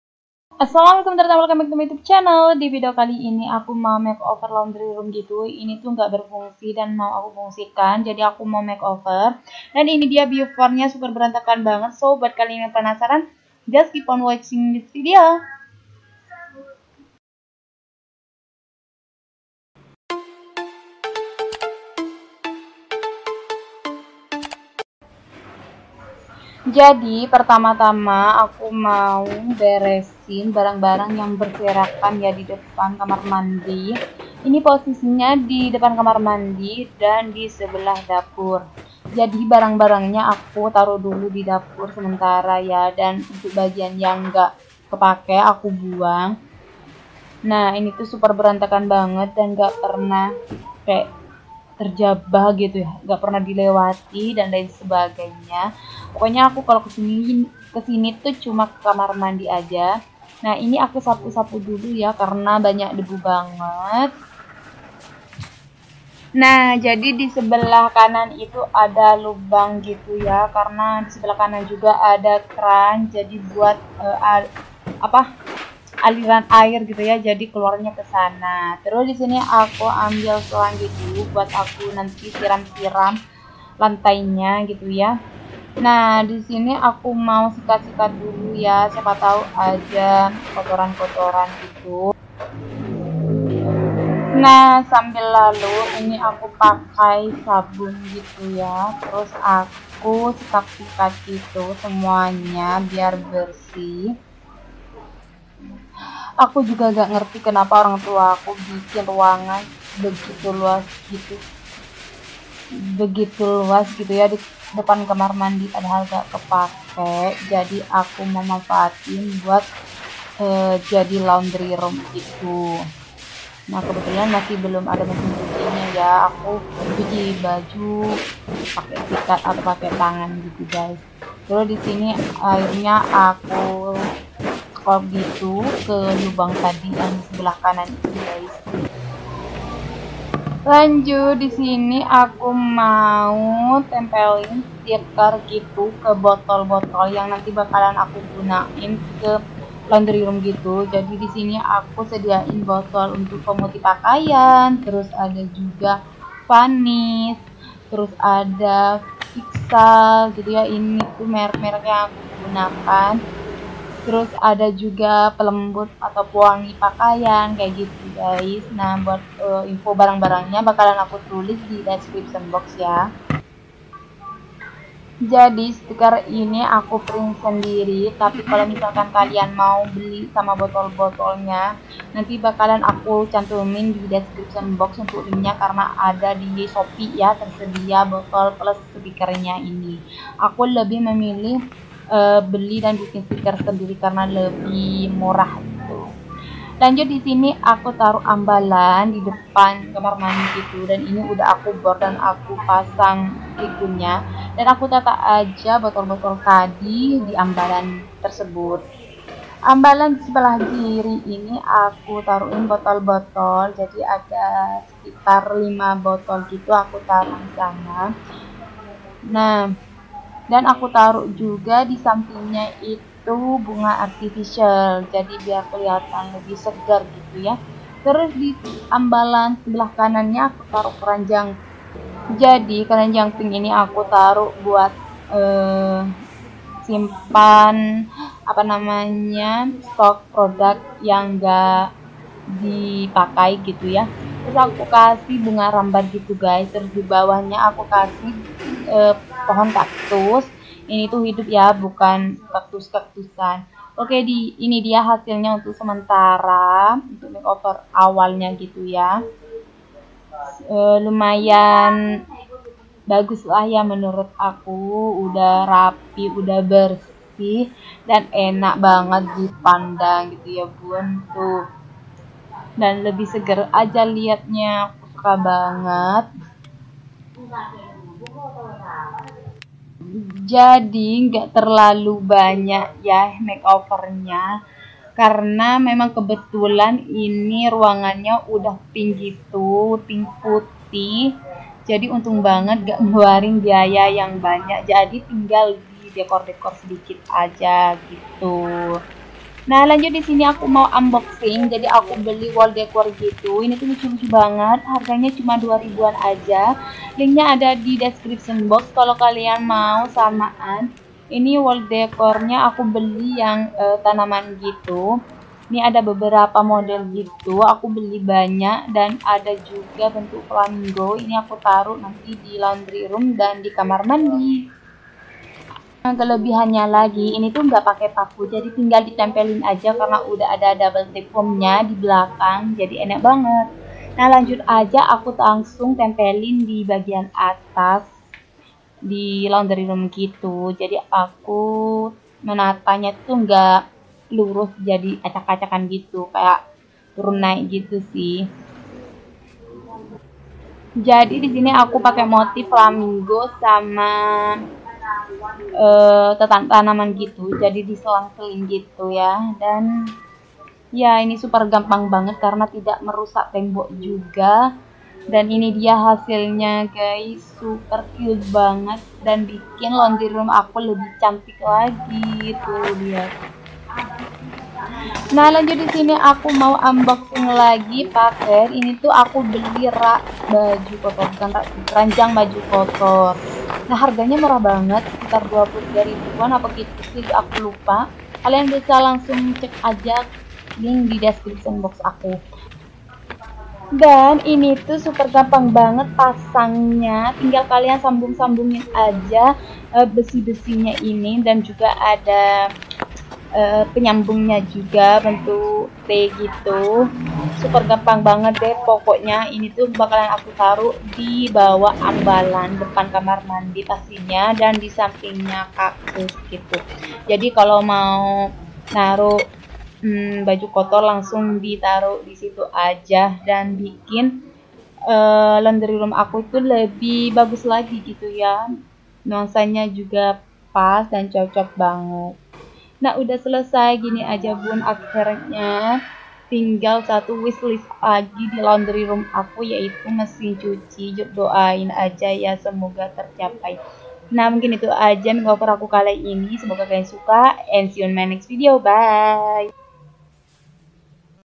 [LAUNDRY ROOM ] EXTREME MAKEOVER LAUNDRY ROOM By Farida FR
Hai semua kali ini aku mau ngerombak laundry room yang emang dasarnya gak kepake dan lumayan luas gitu
Jadi aku pake buat laundry room gitu

7. Hiasan Tulisan laundry Room